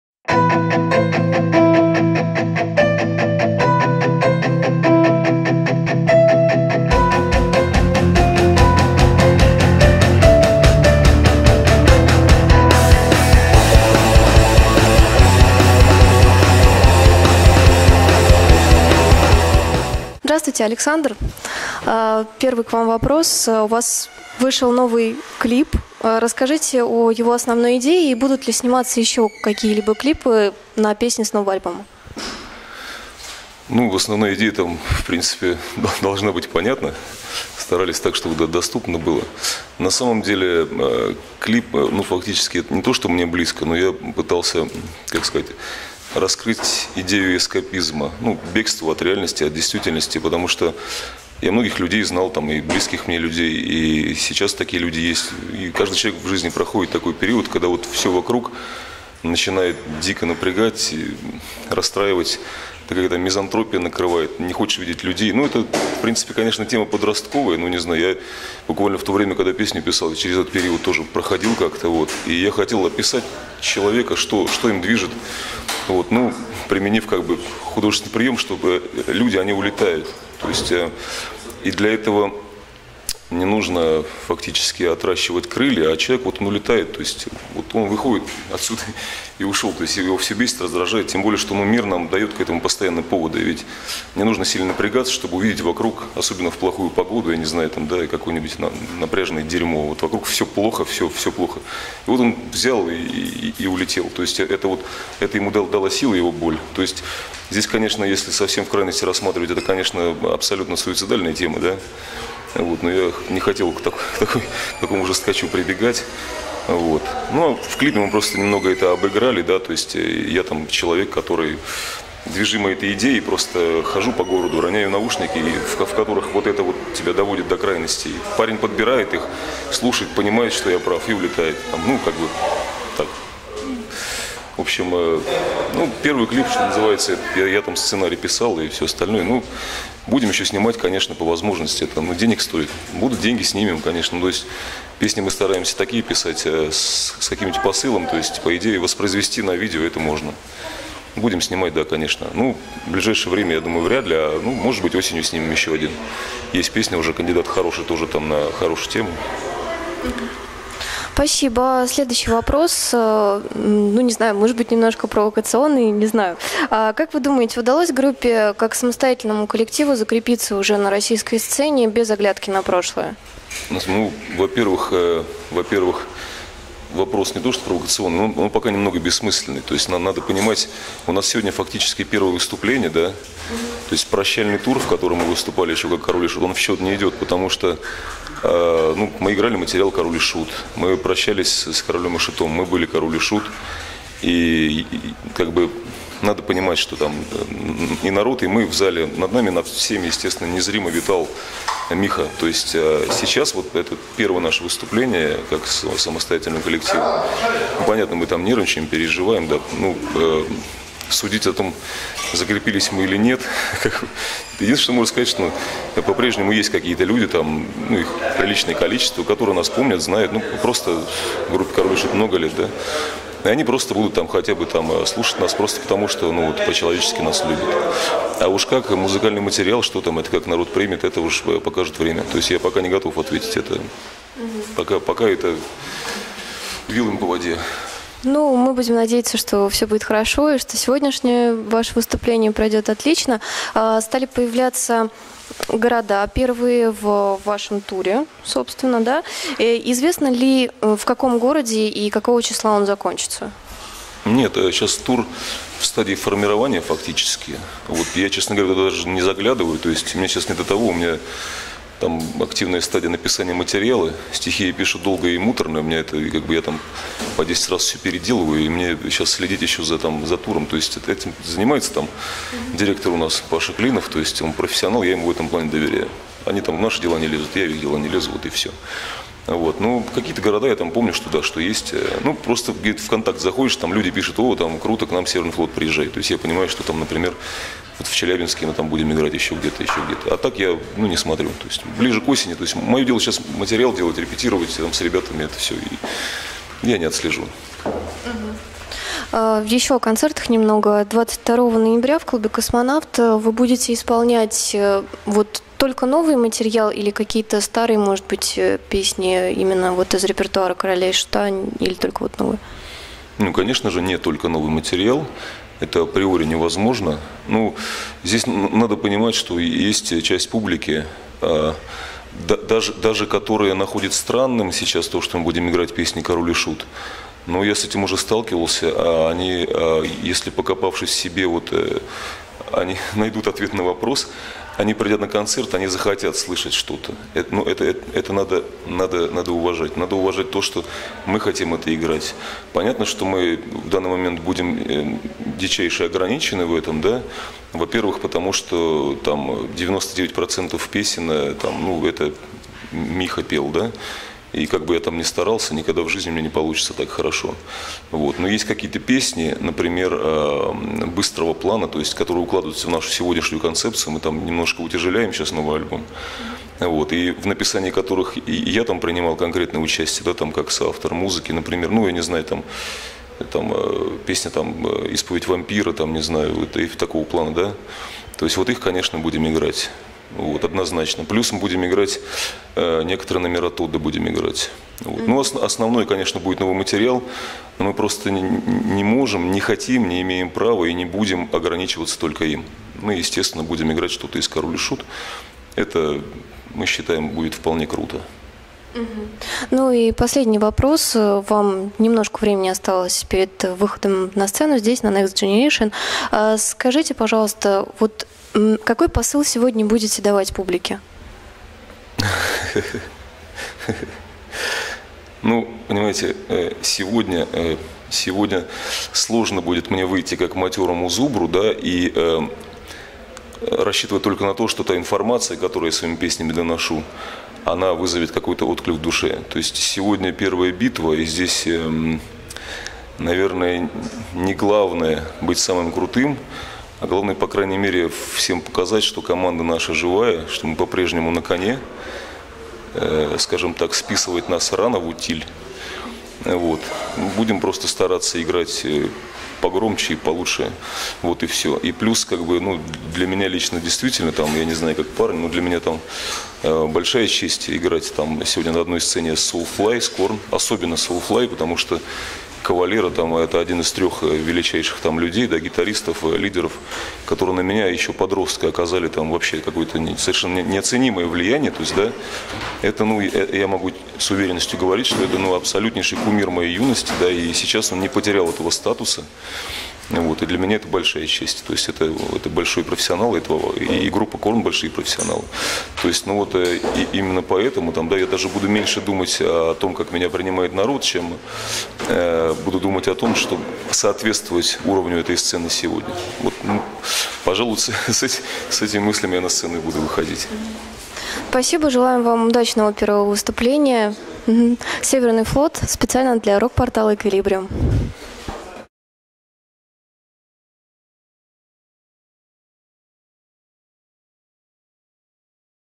Северный Флот. Александр Леонтьев. Интервью Рок-Порталу EQ(24.05.2014)
Интервью Александра "Ренегата" Леонтьева Рок-Порталу EQ, на фестивале Next Generation 2014.

Журналист - Лилия ComEta Фомина
Оператор - Дмитрий Джонни Лихнер